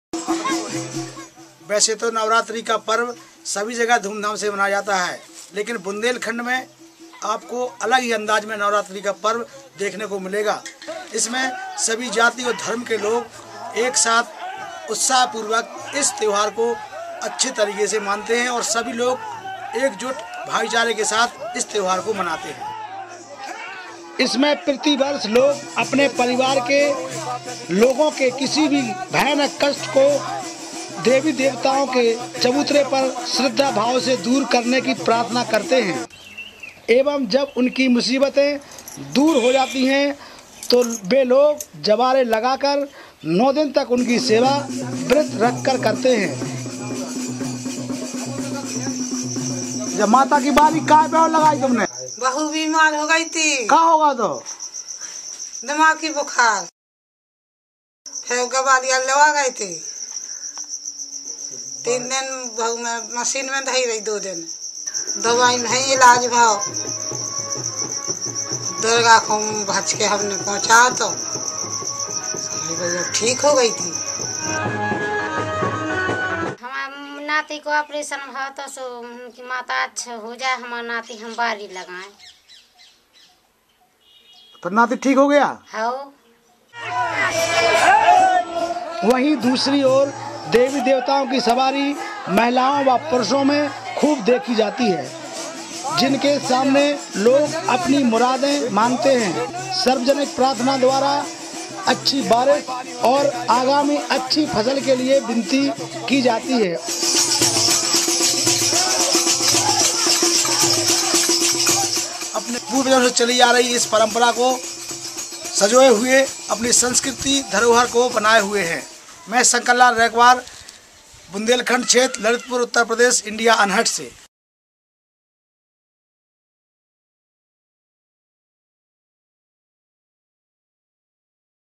Festival of Hope: Navratri in Bundelkhand
For the agrarian community who inhabit the central Indian region known as Bundelkhand, the month of March is the time to raise the hope high. The people have just come out of a hard season, ploughing and sowing at their fields and getting the land ready. Everyone is waiting for the rains to pour. At that moment, comes the auspicious festival of Navratri and the whole region goes ecstatic with hope.

"It is one time in the year when the people can dare to hope for anything and everything under the sun," says native Community Correspondent Shankarlal. "They hope against incurable diseases, against calamities, fate and the odds. The sky is the limit. And you only need to have the belief to do so."

According to the local belief, on the days of the festival, the goddess herself enters the body of the chosen one in the community. The person goes into a dervish trance. The others in village pay their respects and make promises to the goddess in exchange for blessings and miracles.

"You could call it a superstition but it is the culture of the region and the ceremony sets the tone for the festival," says Shankarlal. "The goddess herself has visited the earth to give us her blessings. Once the impossible has happened, everything seems possible."

To propitiate the goddess, the people build 'Ghatts' which are earthen pots stuffed with a local variety of grass. These ghatts are placed at locations in the village considered holy - in the vicinity of the temple, under the banyan tree, in the courtyards of the houses etc. They are allowed to remain there for nine days of the festival and on the ninth night, are set affloat in the nearby ponds, lakes or rivers.

While the most religious and zealous tend to undergo grueling tasks like fasting for nine days or crawling on their knees across the entire village, most believe that the best way to propitiate the goddess to is through song and dance.

"Every night, the people gather in the community space. The nagara and the dholak (drums) are out. Folk songs are ringing through the air. Everybody dances till they can't dance anymore. It's almost like a fever in air. We are so carefree. We are filled with a never-before and never-after wave of hope and joy."

"Those nine days are some of the best of our lives."